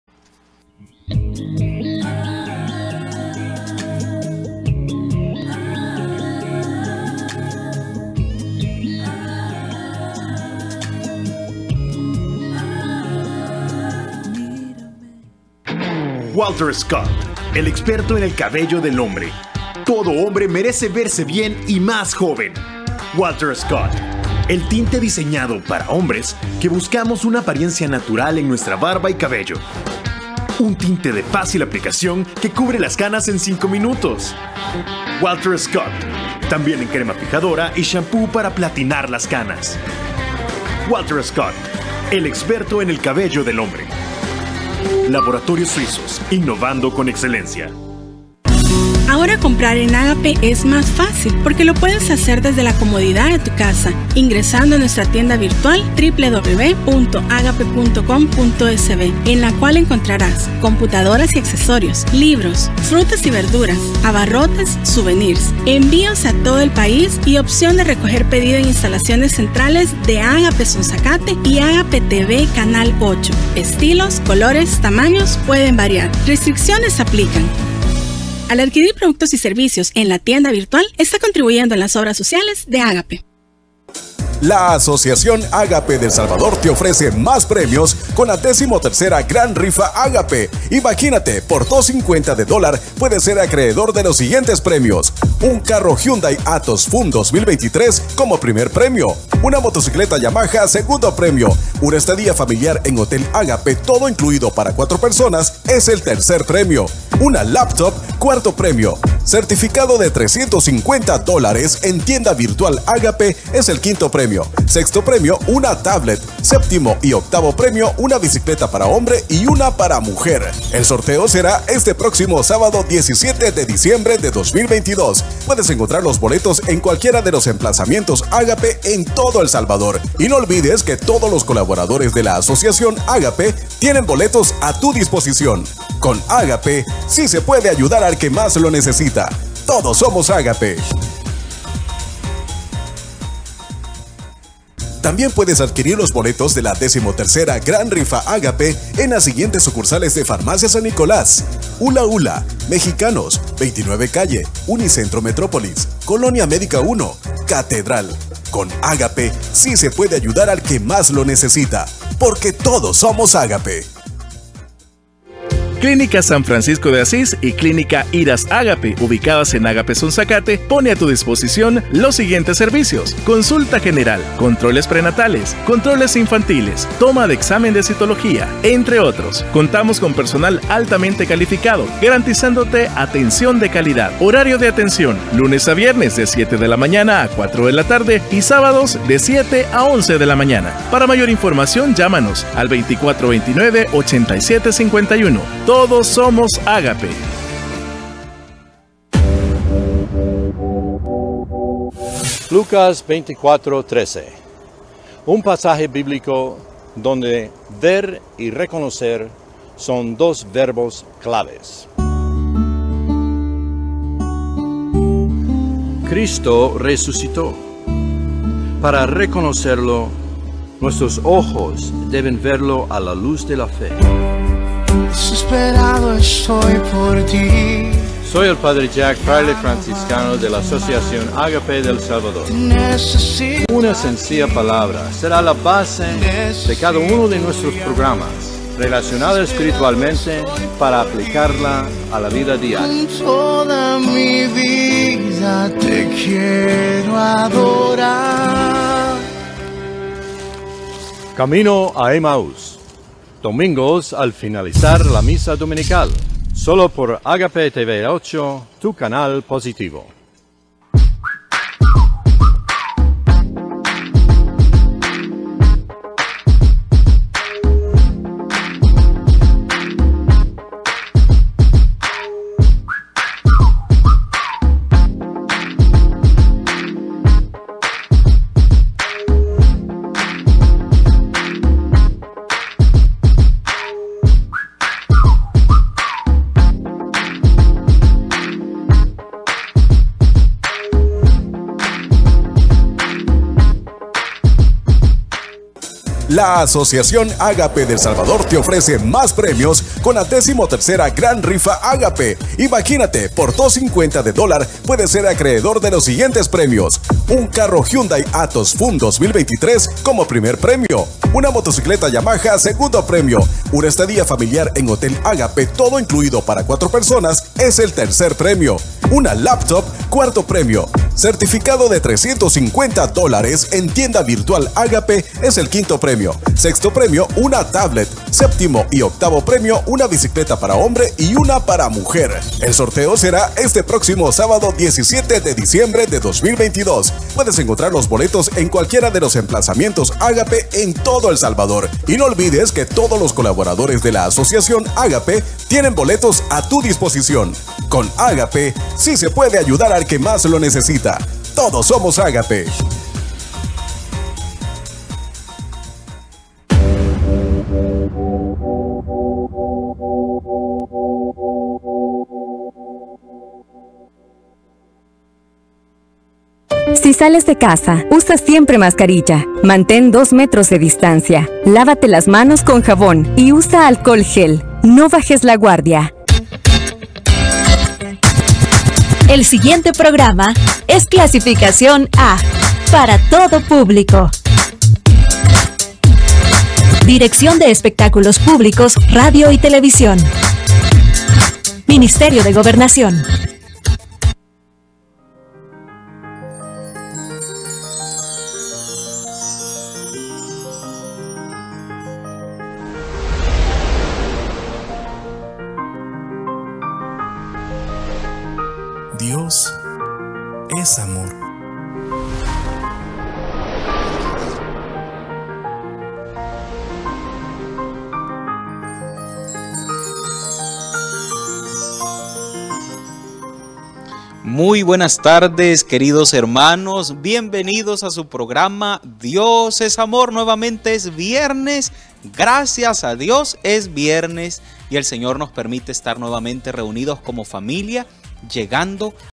Agape TV Canal 8: Tanda Comercial (Octubre 2022)
Al fin logramos un poco de grabacion de Canal 8 Agape TV que hacian falta a.c.a